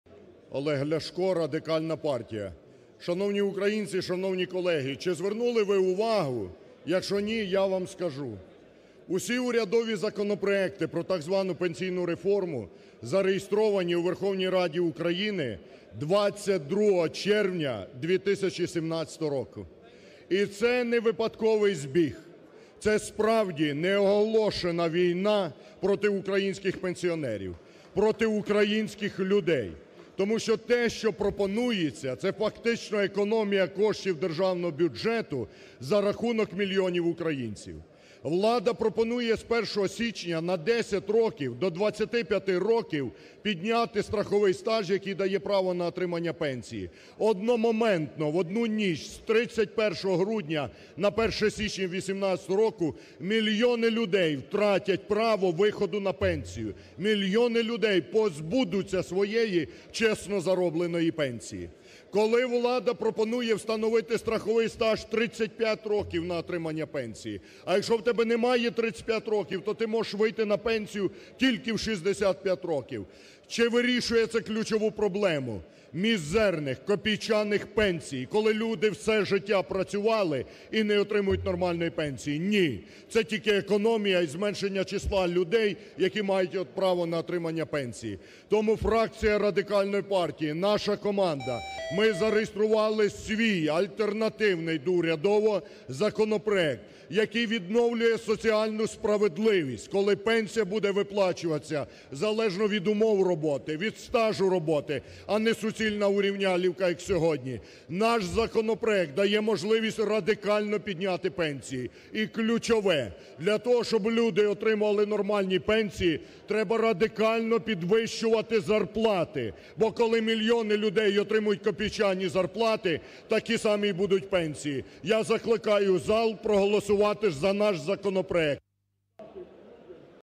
Ляшко про пенсійну реформу уряду: Це неоголошена війна проти українців!
Радикальна партія виступила категорично проти ухваленої у першому читанні так званої пенсійної реформи від уряду. Ляшко назвав запропоновані зміни неоголошеною війною проти українців.
«Усі урядові «пенсійні» законопроекти зареєстровані у Верховній Раді 22 червня 2017 року. Це не випадковий збіг. Це дійсно день початку війни, цього разу – неоголошеної війни проти українців!»,- наголосив Ляшко.
Політик переконаний, що урядова пенсійна реформа переслідує одну мету – зекономити кошти державного бюджету за рахунок українців.
«Влада пропонує на 10 років підвищити трудовий стаж – з 15 до 25 років. В ніч на 1 січня 2018 одномоментно мільйони українців втратять право на отримання пенсії. Разом із цим, у майбутньому влада хоче встановити трудовий стаж на рівні 35 років. А ті, хто його не матимуть, виходитимуть на пенсію у 65 років. І головне, урядова пенсійна реформа не вирішує головної проблеми – подолання бідності», - пояснив народний депутат.
Натомість фракція Радикальної партії зареєструвала альтернативний законопроект, який дозволить відновити соціальну справедливість.
«Пенсія має виплачуватись в залежності від умов та стажу роботи, а не як зараз – суцільна зрівнялівка. Але для того, щоб були високі пенсії, потрібно радикально збільшувати зарплати, адже коли мільйони українців отримують копійчані зарплати, вони отримуватимуть такі ж мізерні пенсії», - розповів Ляшко.